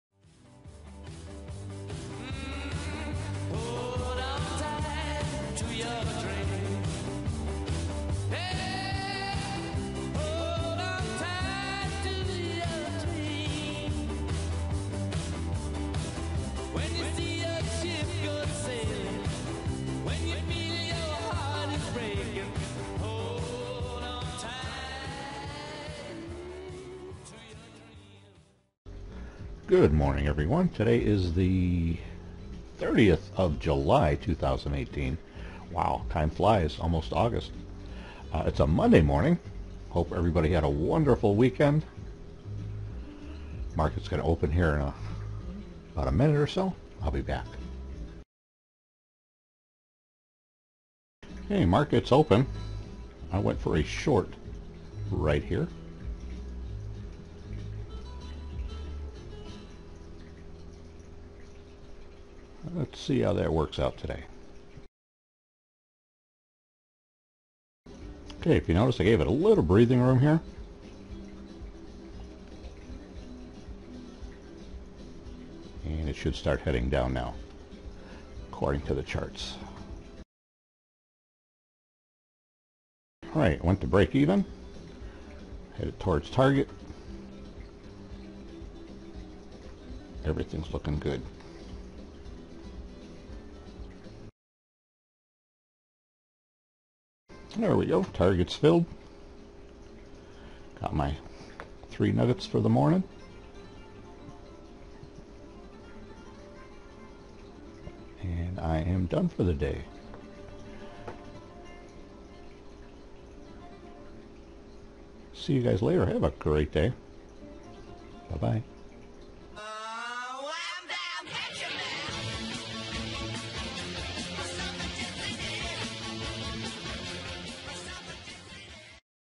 Price Is Going Up Again? Look For Shorts
Price Is Going Up Again? Look For Shorts. Horst Trades RTY July 30th, 2018. How much time do you spend sitting in front of your computer trading EMini Futures on an average day? Possibly even “trend trading”? Trading all day long is a sign of confusion and imminent near-term account failure.